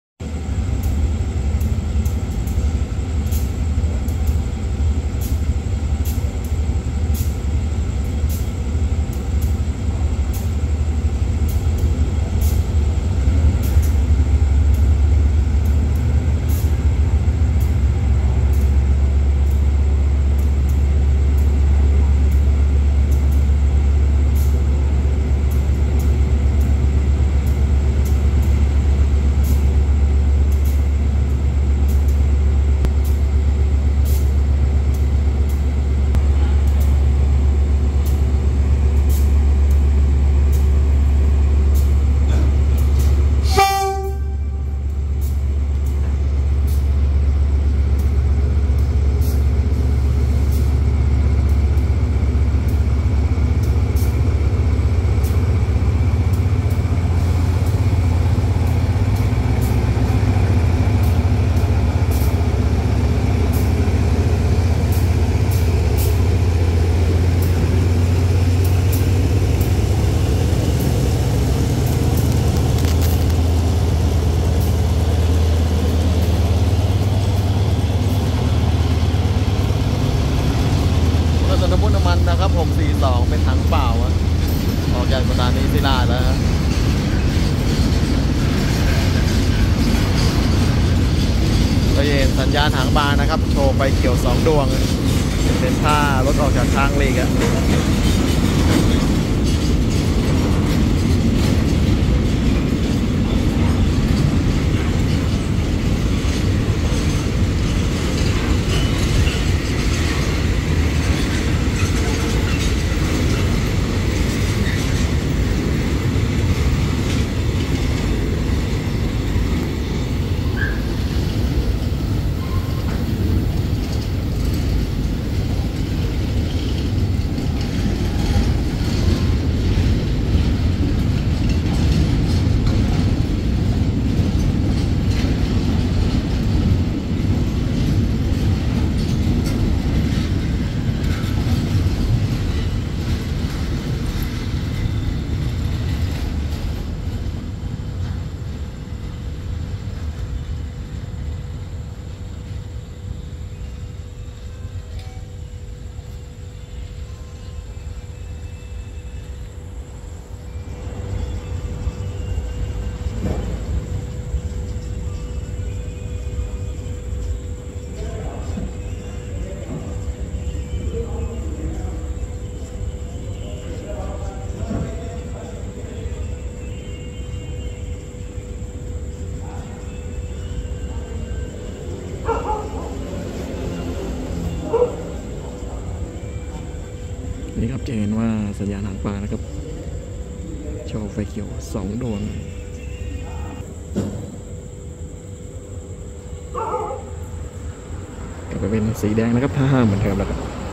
รถน้ำมันถังเปล่า 642 ออกจากศิลาอาสน์ 23 สิงหาคม ค.ศ. 2024
freight train 643